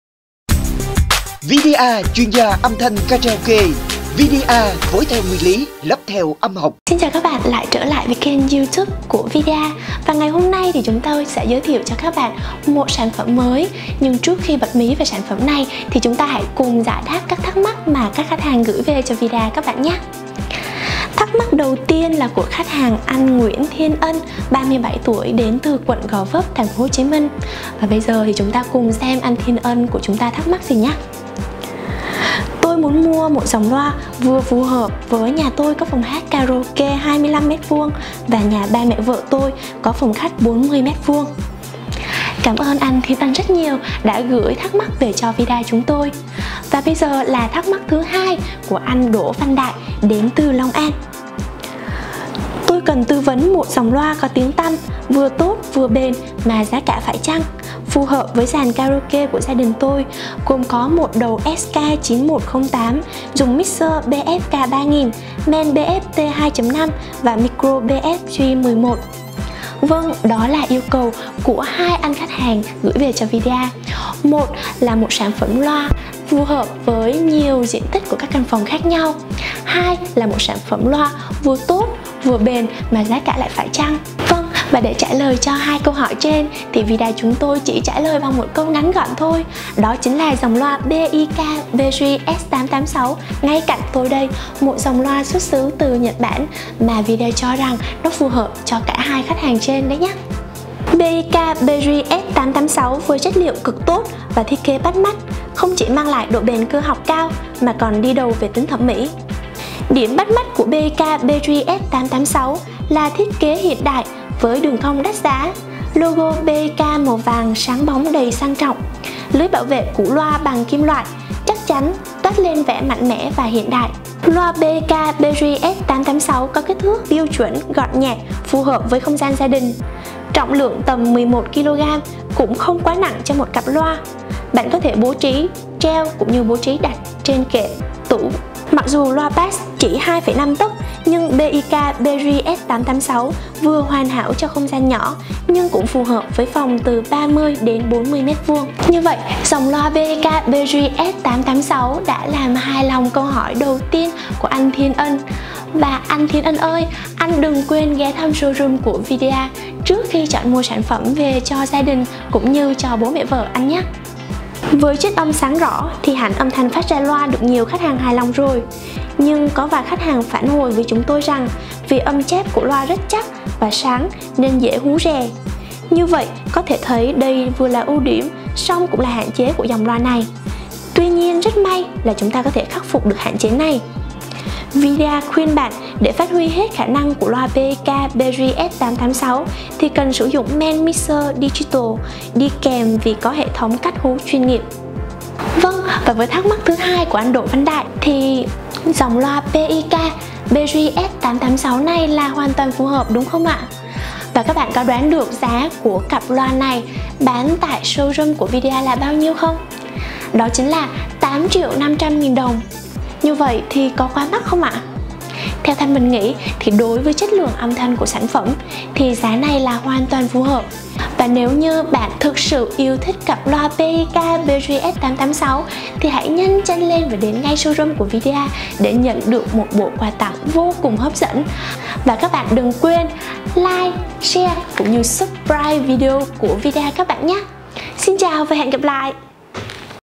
Loa BIK BJ S886: Loa karaoke gia đình hay nhất với thiết kế lạ mắt còn chất âm thì quá đỉnh!
207 Tân Phước, P.6, Q.10, TP.HCM